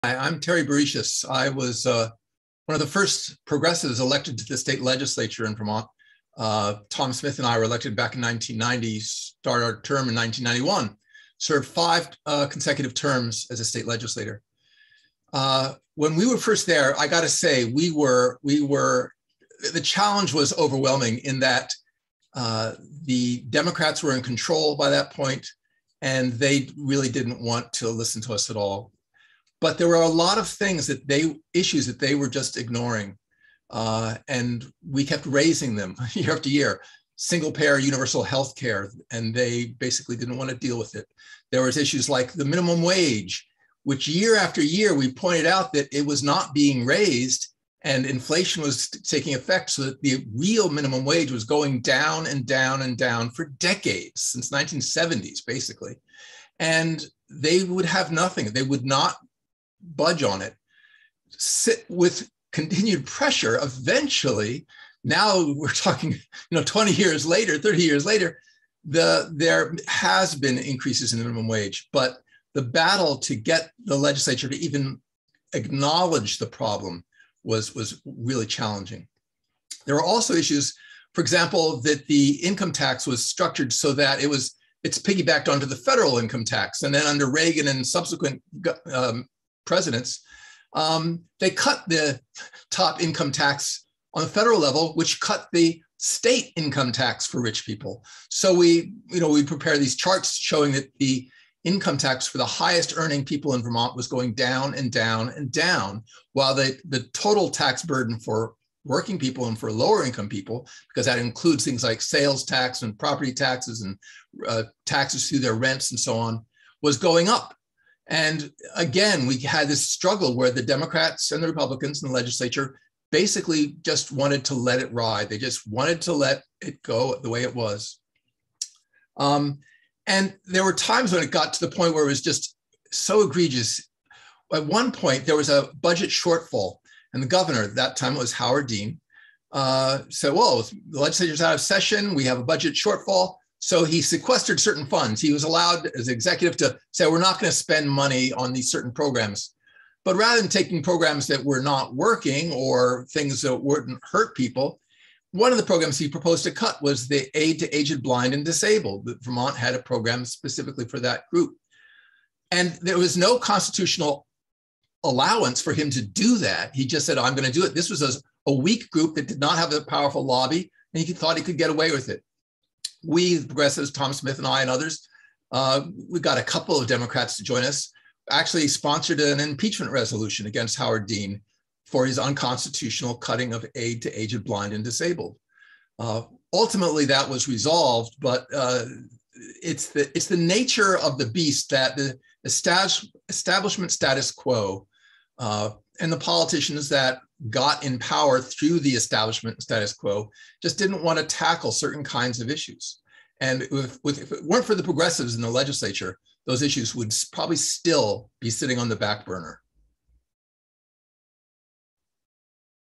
Rep. Terry Bouricius discusses the effects of having Progressives in the VT Legislature
One of two of the first Progressives elected to the VT House talks about the impact of their presence.